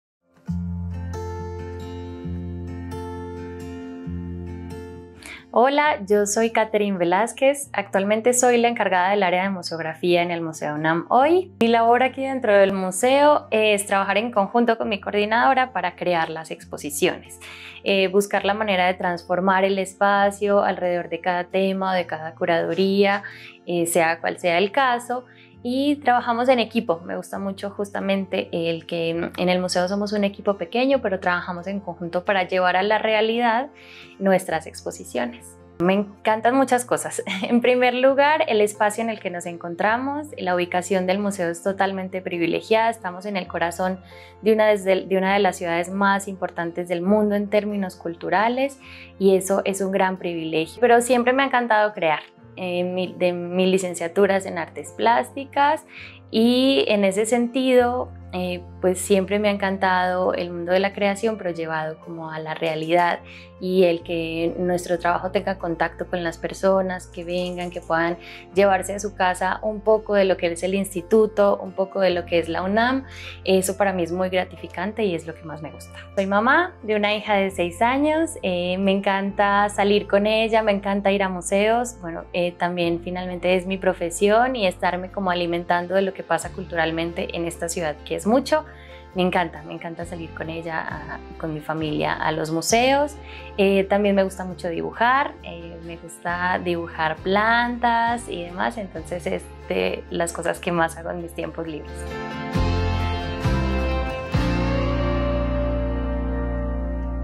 Conócenos: Katherine Velásquez
Creatividad, amor por el arte, la museografía y el trabajo en equipo, hoy en Conócenos, Katherine Velásquez, museógrafa en el Museo UNAM HOY